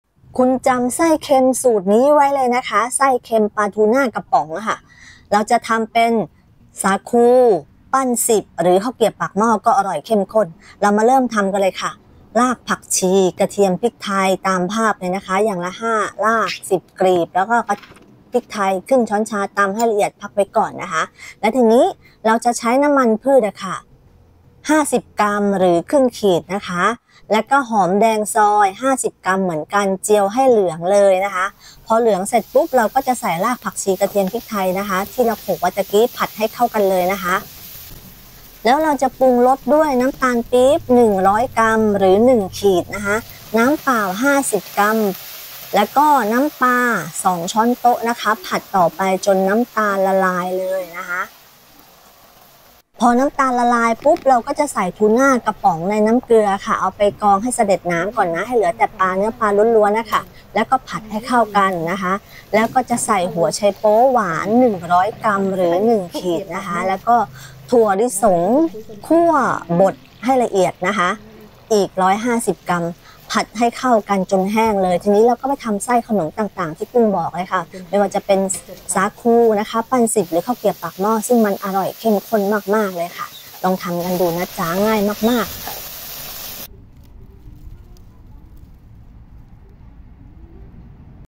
ไส้เค็ม ทูน่ากระป๋อง ไว้ทำ สาคู, ข้าวเกรียบปากหม้อ หรือ ปั้นสิบ
สูตร ตัวไส้
1.  รากผักชี 5 ราก , กระเทียม 10 กลีบ, พริกไทย 1/2 ช้อนชา
2. น้ำมันพืช  50 กรัม
3. หอมแดงซอย  50 กรัม
4. น้ำตาลปี๊บ  100 กรัม
5. น้ำเปล่า   50 กรัม
6. น้ำปลา 2 ช้อนโต๊ะ
7. ทูน่ากระป๋อง 1 ป๋อง พักให้สะเด็ดน้ำ ได้เนื้อสุทธิ 120 กรัม
8. หัวไชโป๊หวาน 100 กรัม
9.ถั่วลิสงป่น 150 กรัม